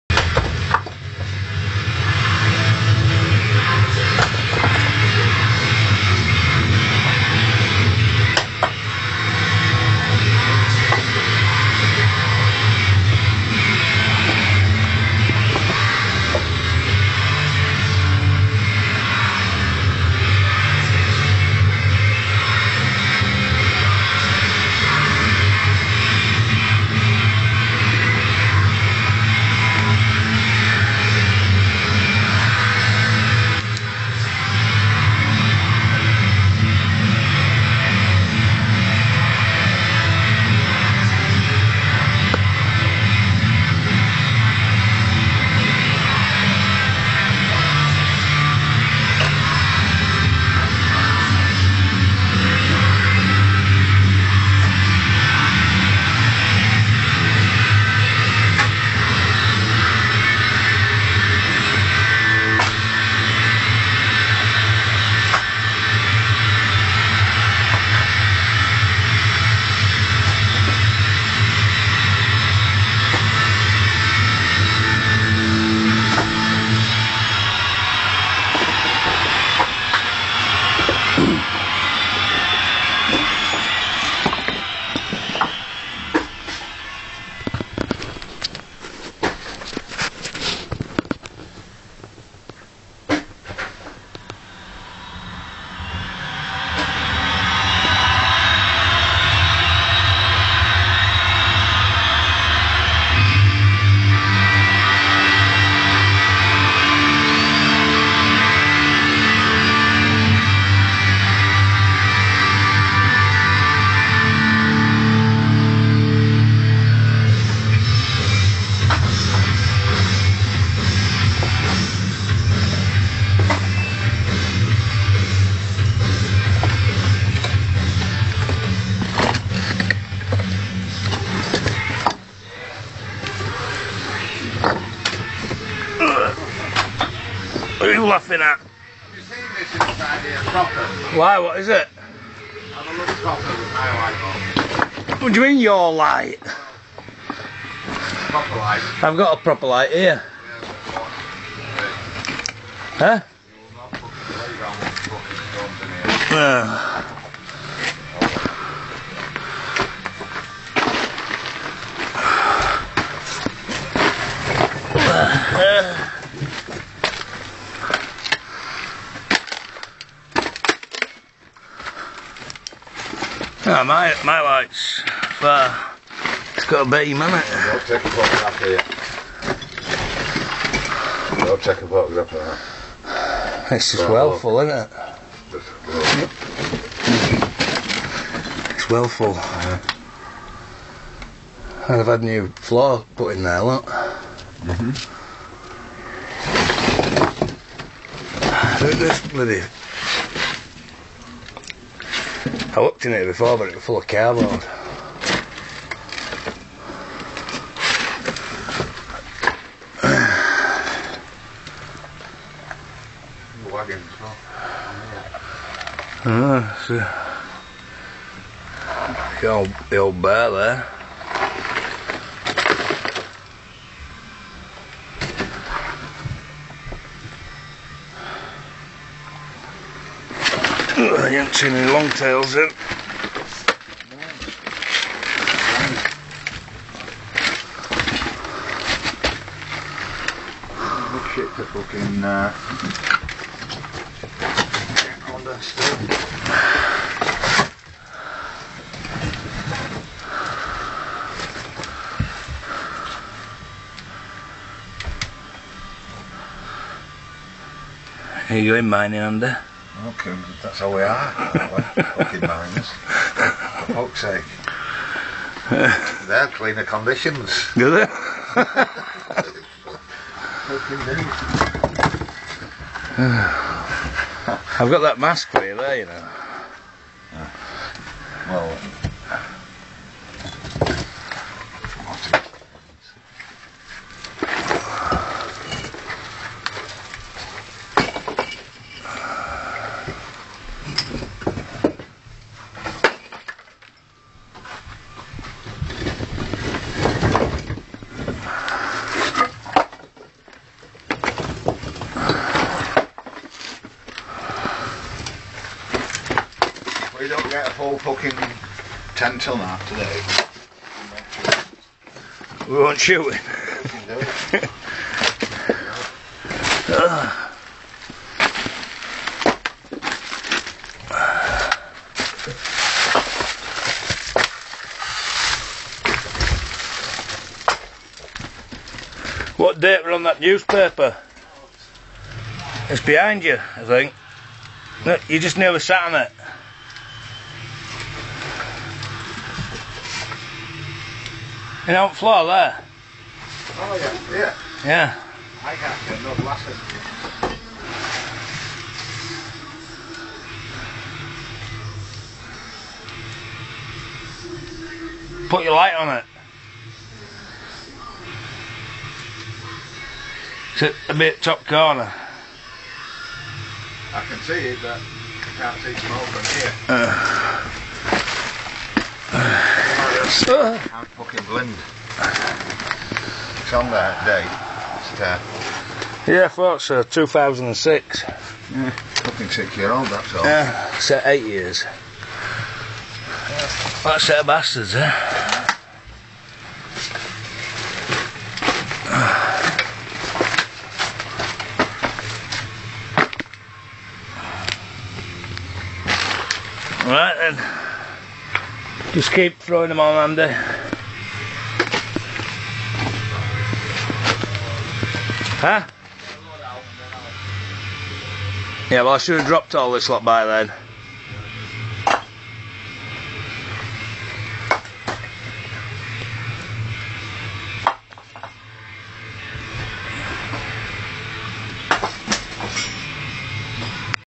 Andy The Miner
Footage from 06 March 2014 of the secret underground tunnel system beneath Bradford City Centre, which will be transformed into Bradford's first underground market. Aimed at 18 to 30 years to create their own businesses at affordable rents with an easy in and easy out status.